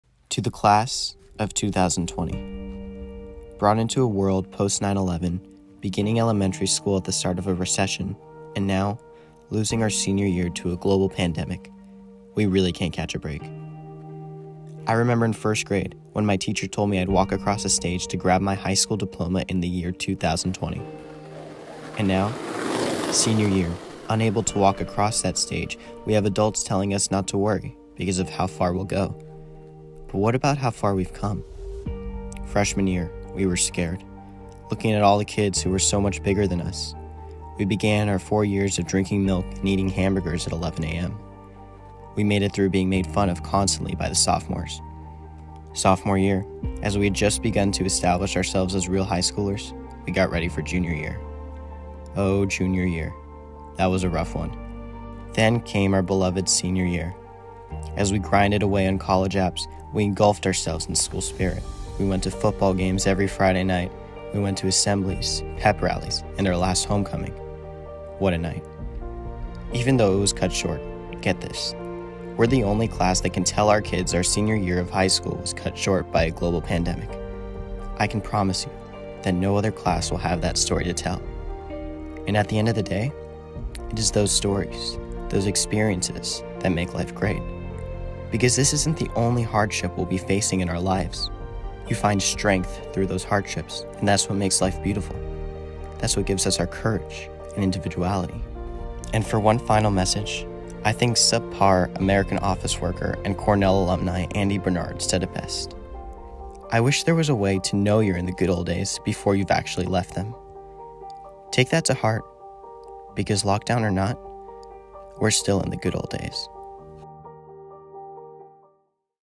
For the Class of 2020 | Doritos Grad Voices Winner
My submissions for the Doritos Grad Voices contest that won the $50,000 prize. My speech was also featured on the iHeart Radio 2020 Commencement podcast alongside Hilary Clinton, John Legend, DJ Khaled, and other celebrities. I hope this video helps my fellow seniors look on the bright side of things.

Scroll to "Doritos Valedictorian Speeches" to hear my speech.